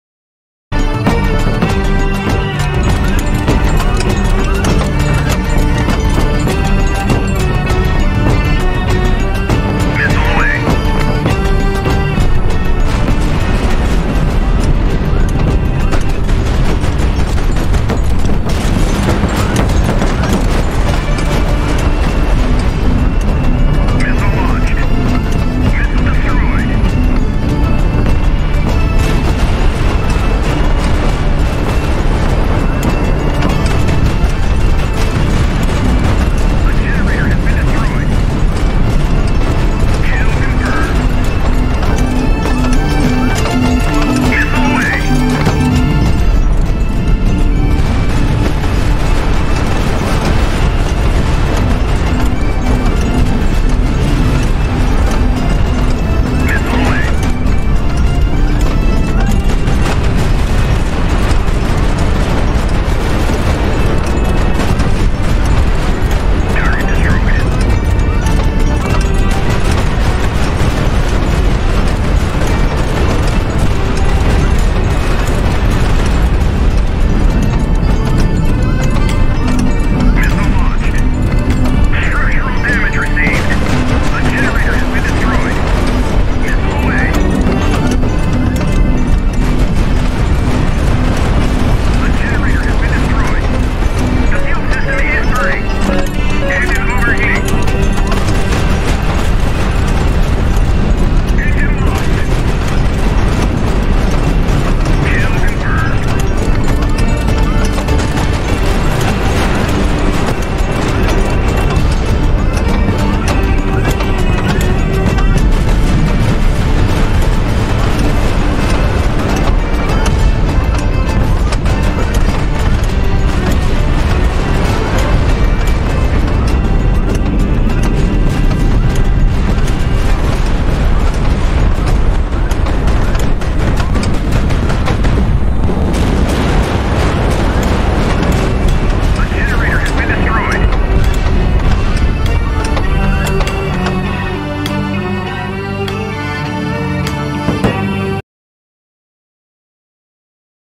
Down, but not out.
cruise missiles took out most of the fleet and crippled my flagship. Flagship standing alone against a cruiser strike group.